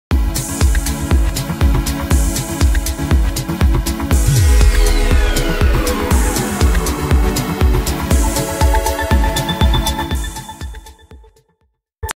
Troy Pallas Instrumental Music Dance Stinger 12 seconds
Troy Pallas Dance Music Stinger 12 seconds for podcasts intro/outro available from Pond 5